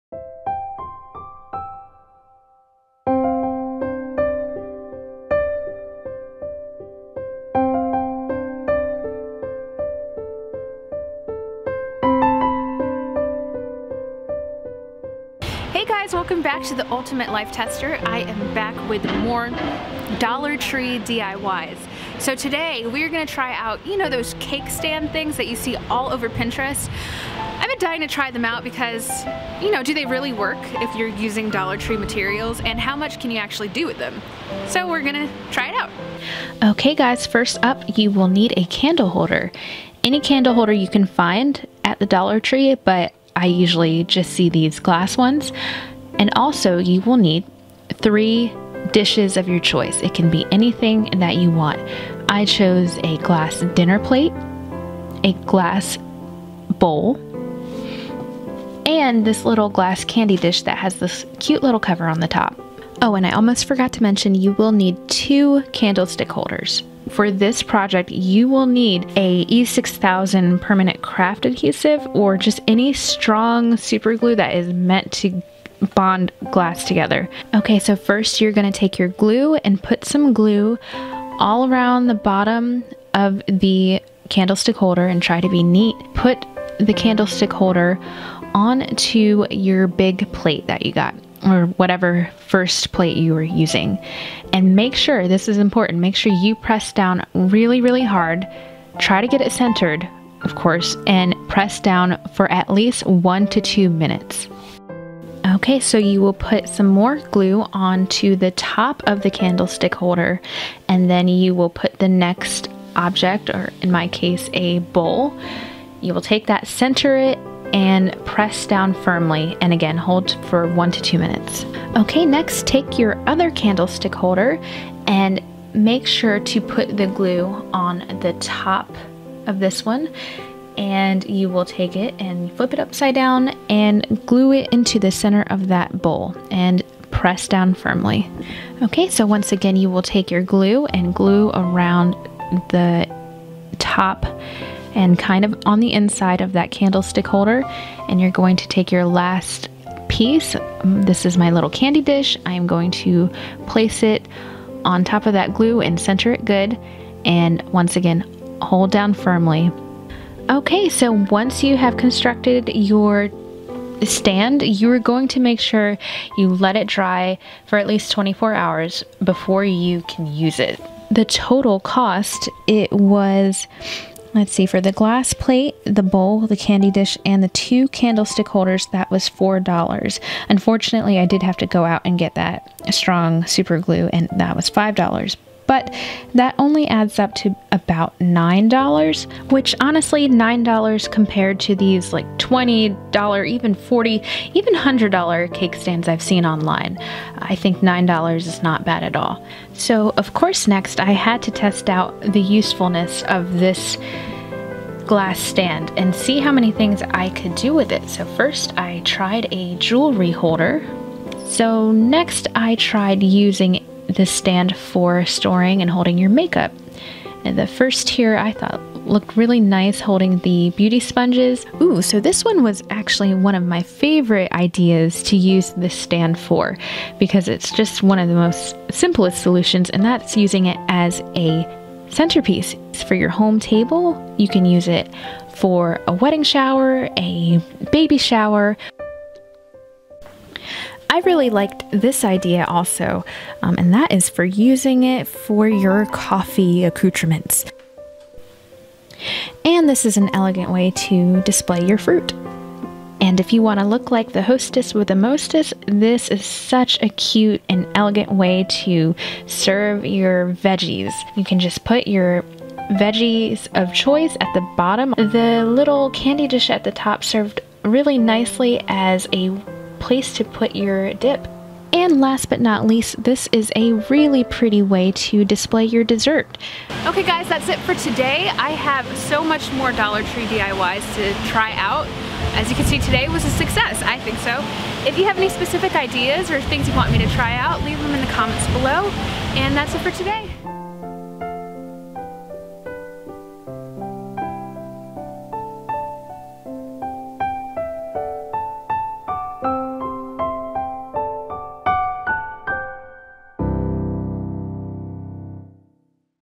Dollar Tree DIY 3 Tier Stand|Ultimate Life Tester
Easy 3 Tier Stand from the Dollar Tree!

Hello everyone! Testing out a popular pinterest inspired DIY. All materials except for the glue is from the Dollar Tree!